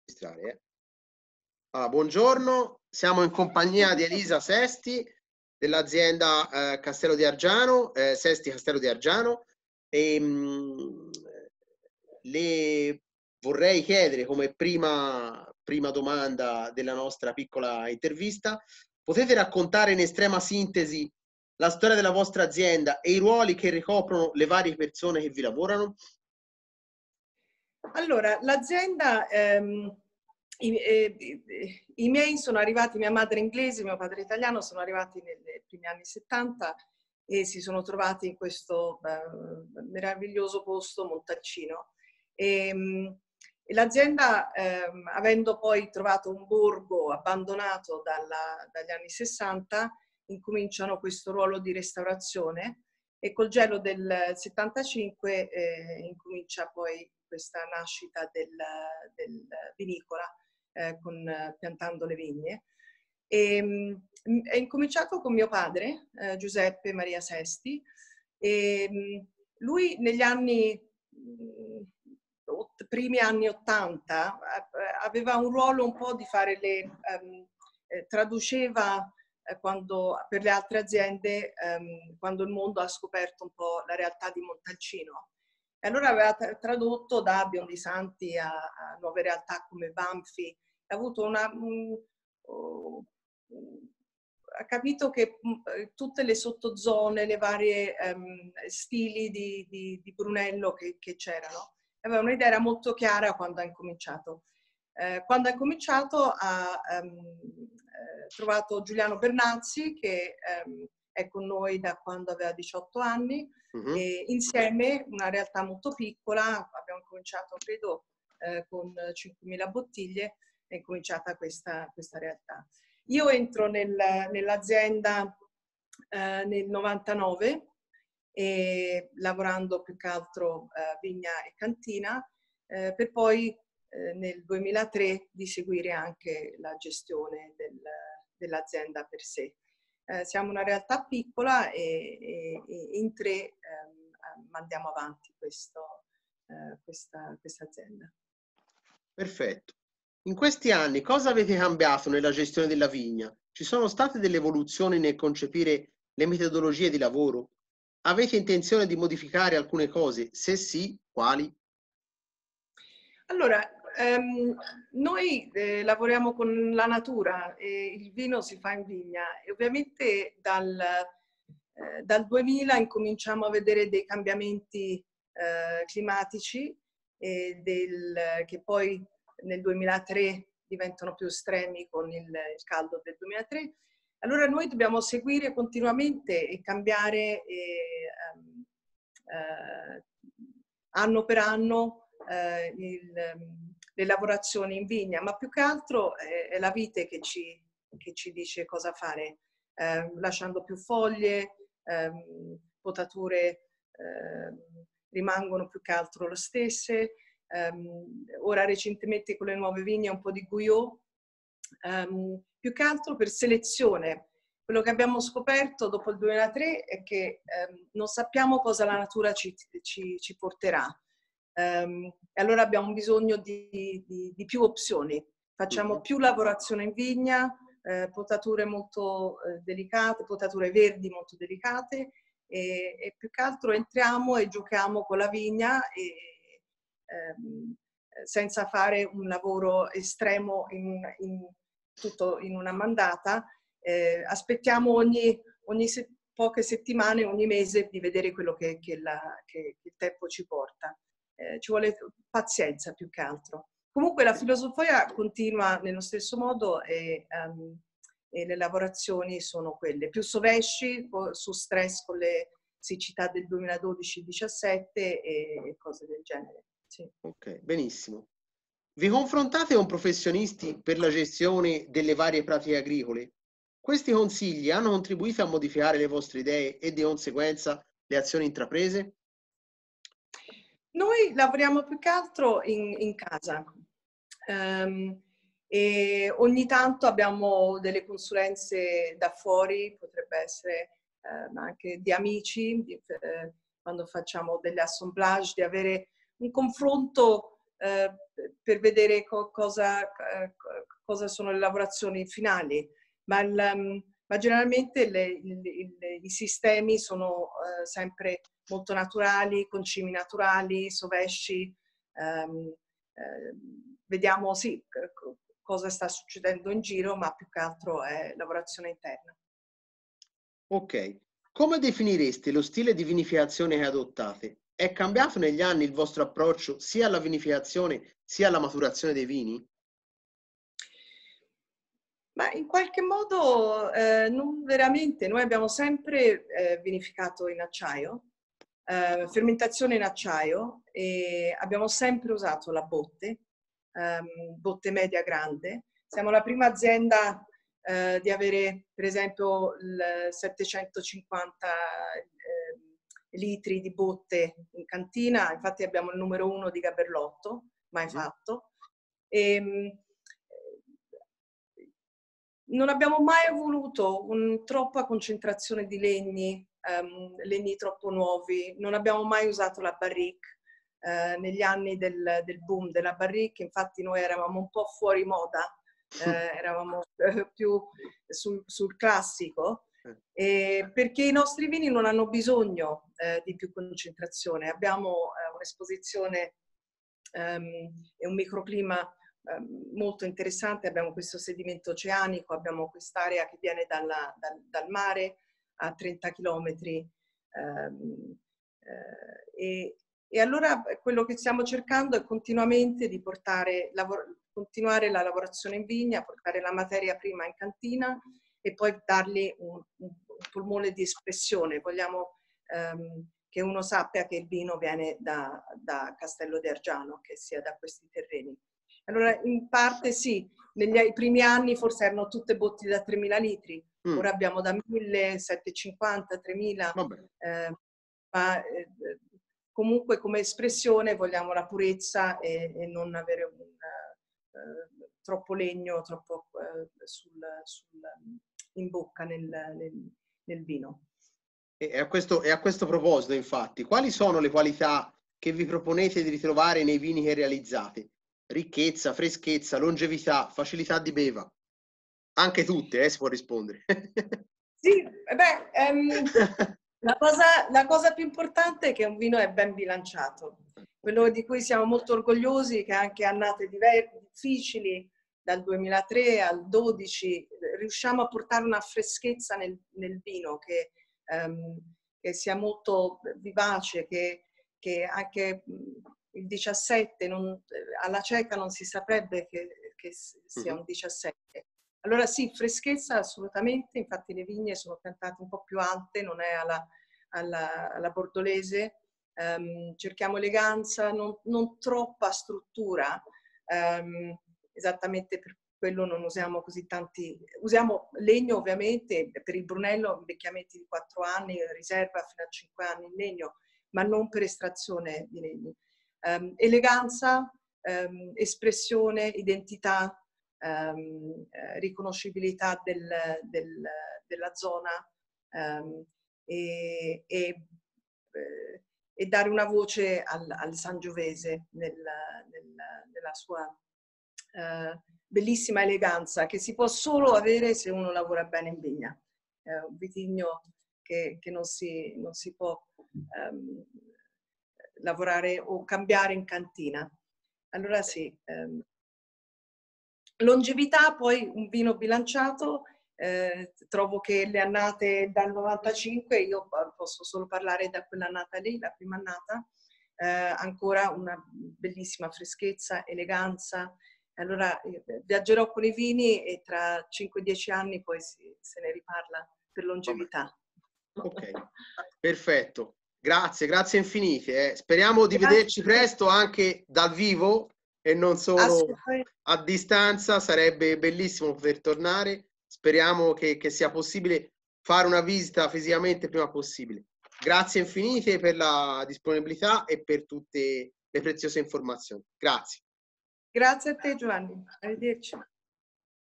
Sesti - Castello di Argiano - Slow Wine 2021
Visita virtuale per la guida Slow Wine 2021 presso l’azienda vitivinicola Sesti - Castello di Argiano. Intervista a cura di Giovanni D'Alessandro.

Sesti - castello di Argiano
Montalcino (Si)
Toscana